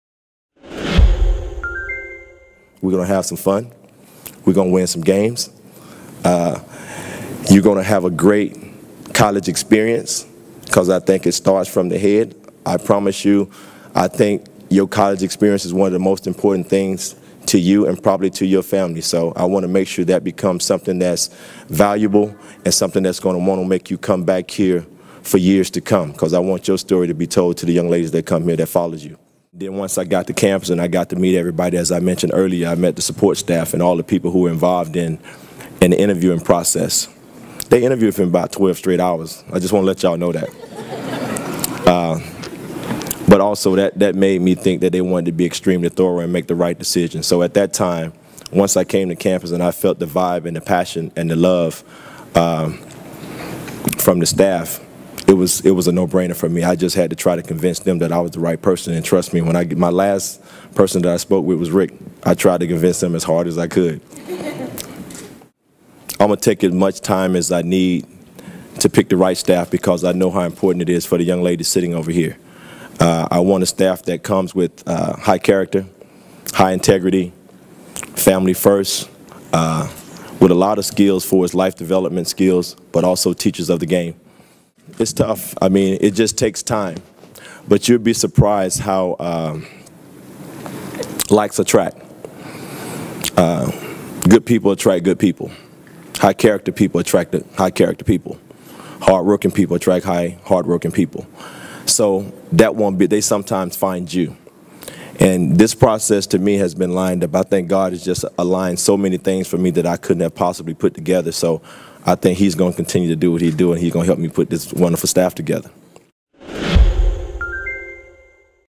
SMU Introduces Travis Mays As New Head Women's Basketball Coach
Travis Mays answers questions after being introduced as head coach of the women's basketball team at SMU in Moody Coliseum in Dallas Friday April 8, 2016. (DMN Video/editing: Andy Jacobsohn)